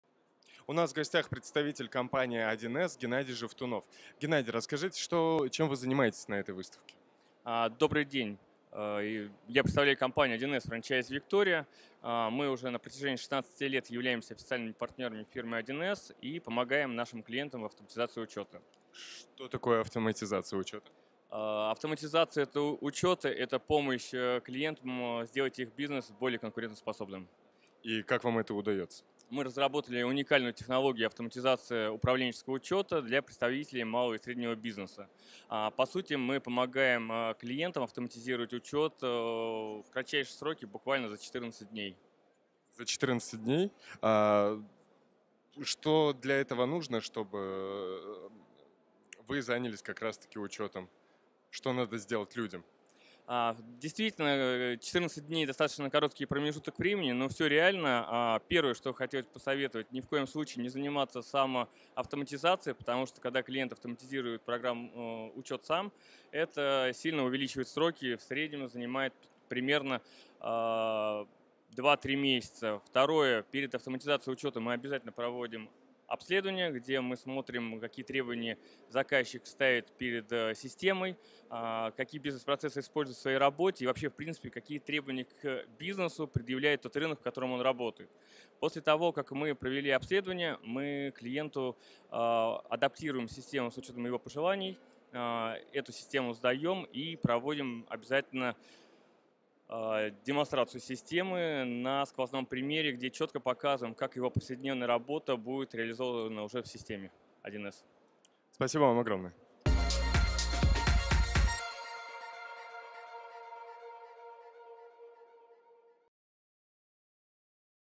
Геннадий Жевтунов, 1С
Геннадий Жевтунов, 1С, рассказывает об уникальной технологии автоматизации учета, разработанной компанией для представителей малого и среднего бизнеса.